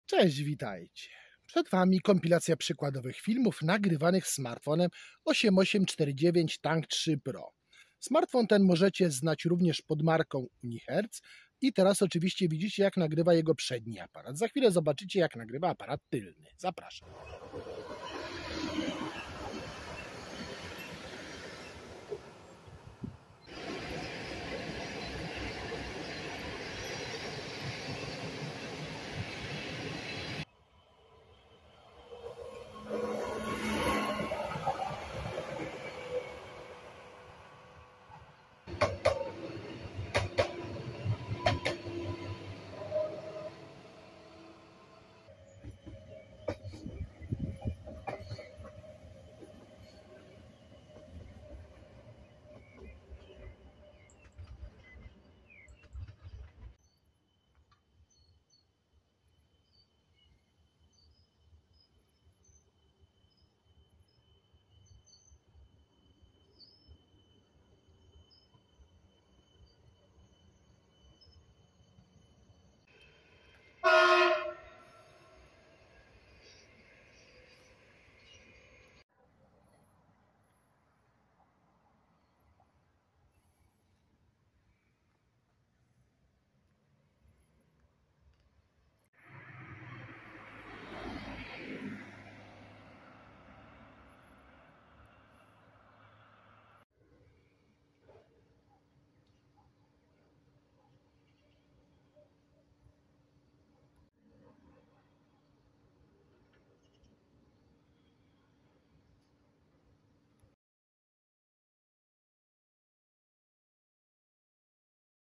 8849 Tank 3 Pro - przykładowe filmy
Kompilacja przykładowych filmów nagranych smartfonem Unihertz 8849 Tank 3 Pro.

8849 Tank 3 Pro szokuje niemal w każdym aspekcie - od wyglądu, poprzez możliwości, aż po cenę. Cóż takiego ma 8849 Tank 3 Pro?
Szczerze mówiąc, łatwiej byłoby mi wymienić, czego w  8849 Tank 3 Pro nie znajdziemy... Chyba tylko wodotrysku! Zaczniemy od podstaw, które w tym smartfonie są najmniej zaskakujące. Urządzenie oparto o procesor MediaTek Dimensity 8200 oraz 6,79-calowy wyświetlacz Full HD+ o częstotliwości odświeżania obrazu 120 Hz. Na tle reszty specyfikacji, bardzo marnie prezentuje się wysłużony system Android 13 - ale to chyba najsłabszy punkt specyfikacji. dalej jest lepiej, mamy bowiem do wyboru pamięci 16 lub 18 GB RAM i 512 GB na dane. Jest też 18 GB wirtualnego RAM-u oraz slot na kartę microSD do 2 TB.
Fotograficznie także jest niebiednie - przynajmniej "na papierze". W 8849 Tank 3 Pro mamy bowiem 50-megapikselowy aparat do selfie, 200-megapikselowy aparat główny, a także 50-megapikselowy aparat szerokokątny i 64-megapikselowy aparat nocny, noktowizyjny. Obudowa smartfonu jest oczywiście wzmocniona, spełnia wymogi szczelności przed pyłem i cieczą na poziomie IP68. Pomimo szczelności, smartfon wyposażono w wentylator, aktywnie chłodzący wnętrze.  Dalej zaczyna się jeszcze większy odlot. 8849 Tank 3 Pro ma akumulator o pojemności aż 23800 mAh, w dodatku z szybkim ładowaniem o mocy 120 W. To z pewnością wpływa na wymiary oraz wagę telefonu, które wynoszą 179x86x29.8 mm i... 696 g! Na liście wyposażenia znajdziemy także niezbędne moduły łączności, w tym Wi-Fi 6, NFC czy Bluetooth 5.3 oraz GPS L1+L5, ale to nadal nie koniec niespodzianek... Unihertz 8849 Tank 3 Pro ma jeszcze... projektor o sile światła 100 lumenów. Niby niewiele, ale w ciemności da się coś obejrzeć... Cena wynosi 999 dolarów, czyli niecałe 4000 zł.